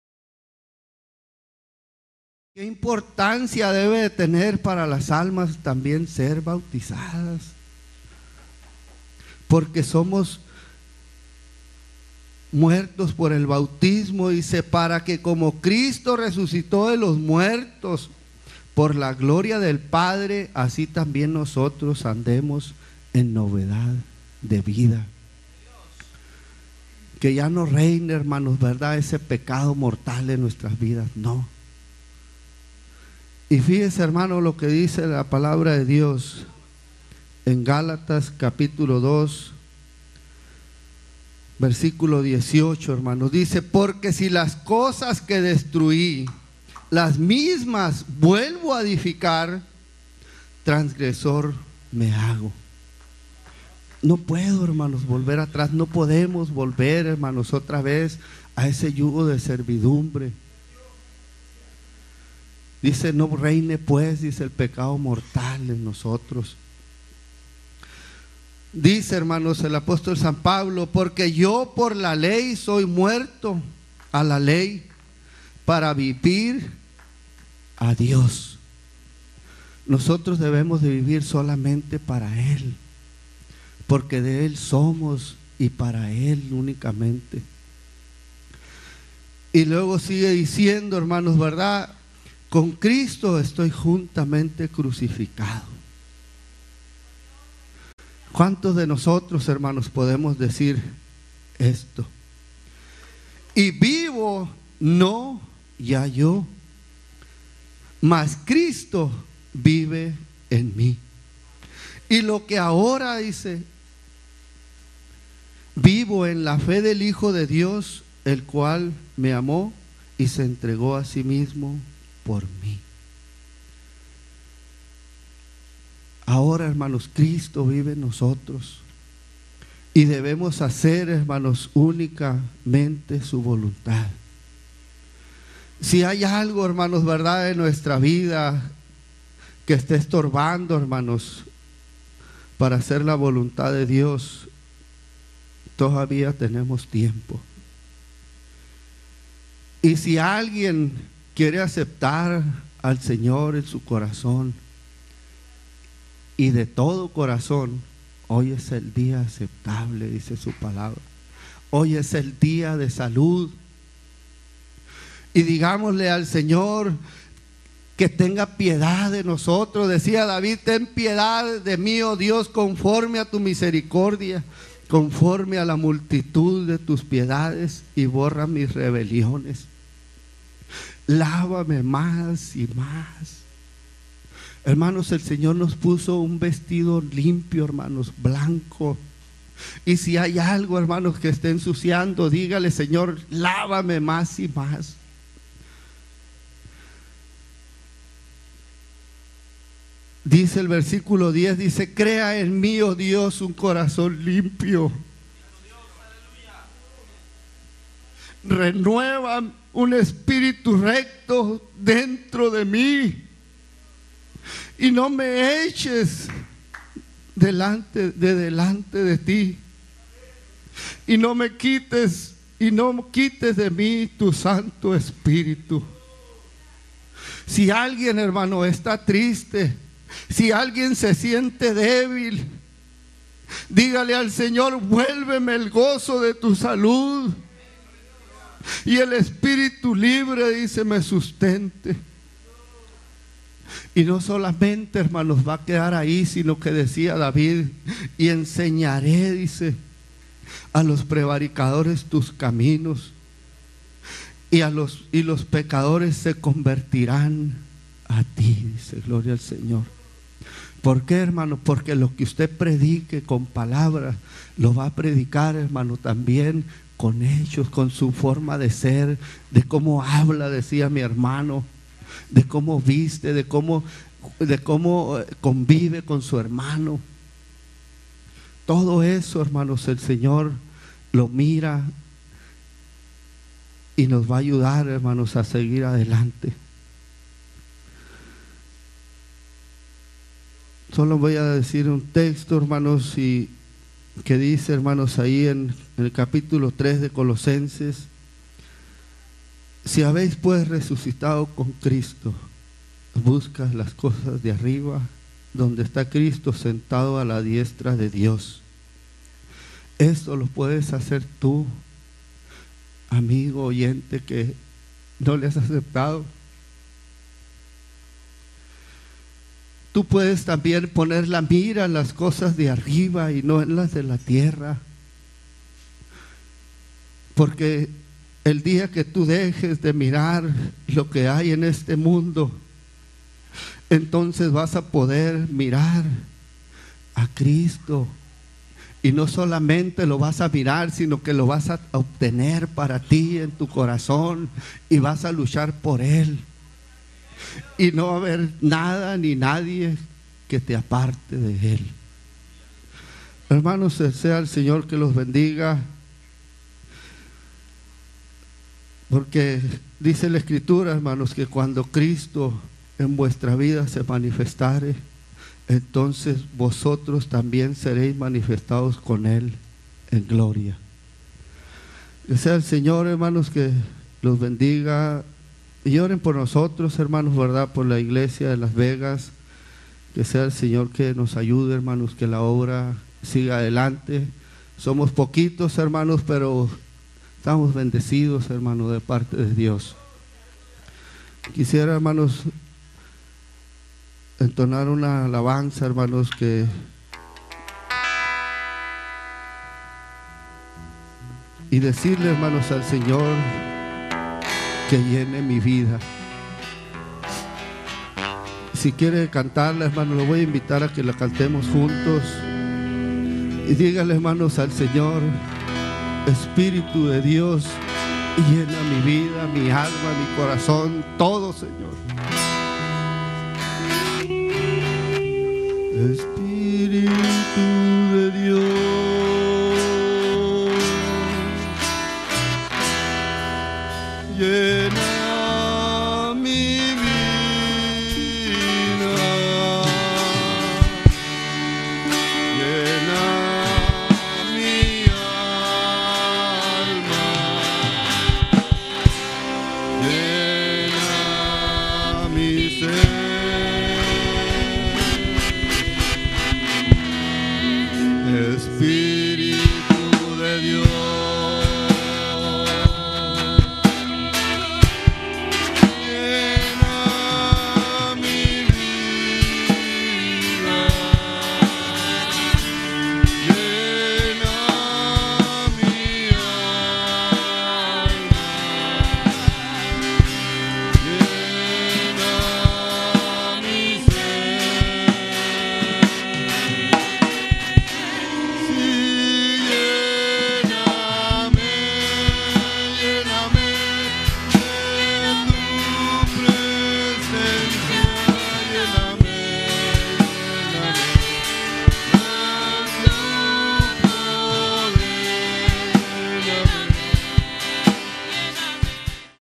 Pastor Uziel Montiel [Bakersfield CA]
Con palabra de Dios, nuestro hermano tambien entona una alabanza.